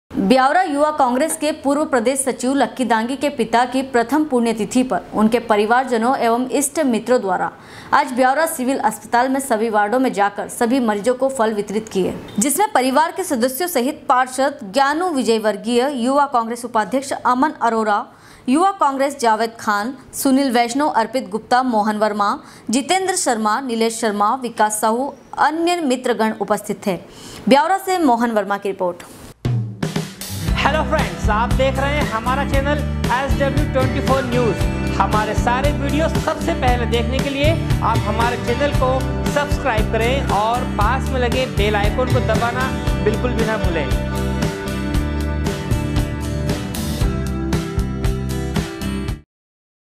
byawara- ब्यावरा युवा कांग्रेस के पूर्व प्रदेश सचिव लक्की दांगी के पिताजी की प्रथम पुण्यतिथि पर
ब्यावरा युवा कांग्रेस के पूर्व प्रदेश सचिव लक्की दांगी के पिताजी की प्रथम पुण्यतिथि पर उनके परिवार जनों एवं इष्ट मित्रों द्वारा आज ब्यावरा सिविल अस्पताल में सभी वार्डाे में जा कर सभी मरोजो  फल वितरित किए। ब्यावरा से मोहन वर्मा की रिपोर्ट